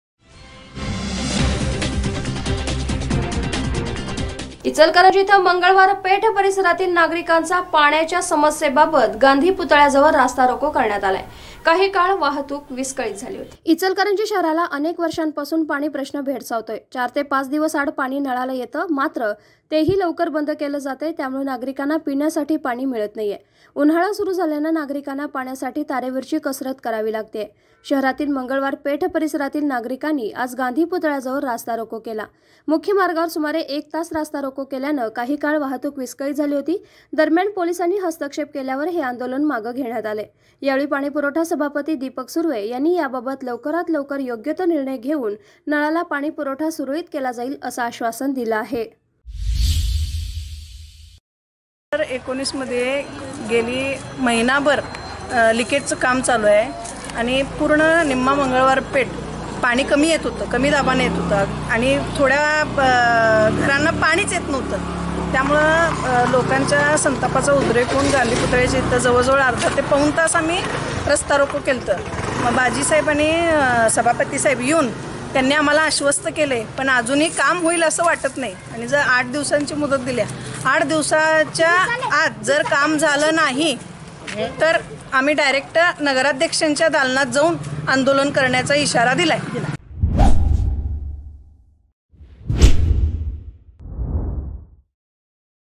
ICHALKARNJI | इचलकरंजी येथे नागरिकांचा पाण्याच्या समस्येबाबत गांधी पुतळ्याजवळ रस्ता रोको
इचलकरंजी येथे मंगळवार पेठ परिसरातील नागरिकांचा पाण्याच्या समस्येबाबत गांधी पुतळ्याजवळ रस्ता रोको.@